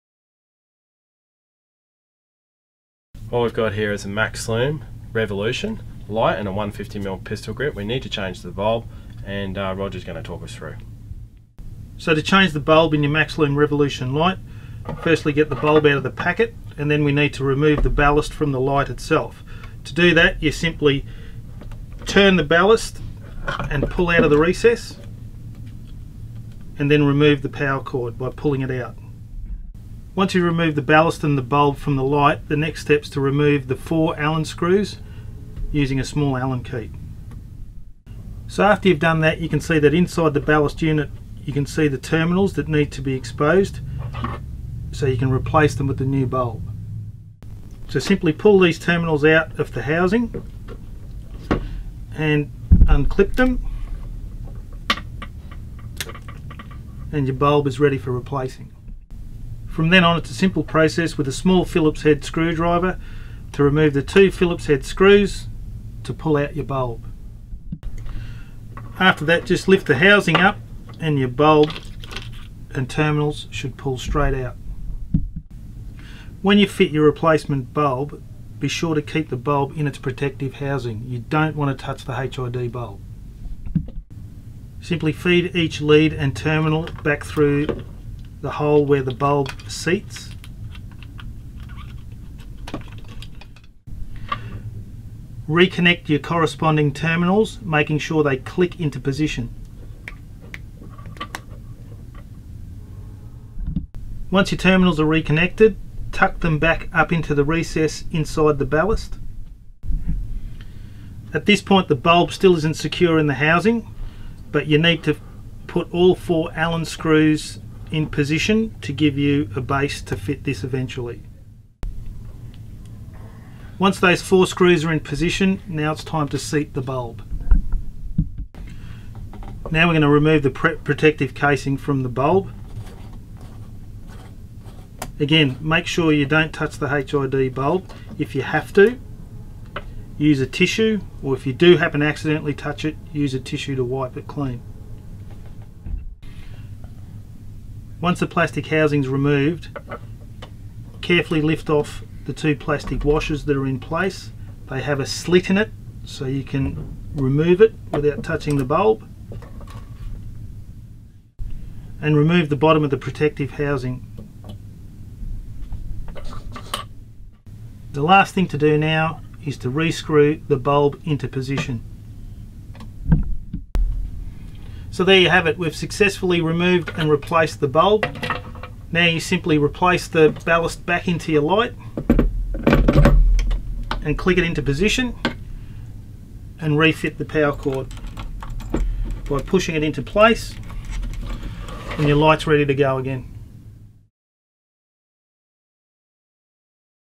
Pro-Tactical Max-Lume Revolution Bulb Replacment
A quick how to on replacing a bulb in the Max-Lume Range of Spotlights.
**Please ensure if you have to handle the bulb**
** DO NOT TOUCH IT WITH YOUR BARE HANDS**
Use a tissue and wipe down the bulb with a tissue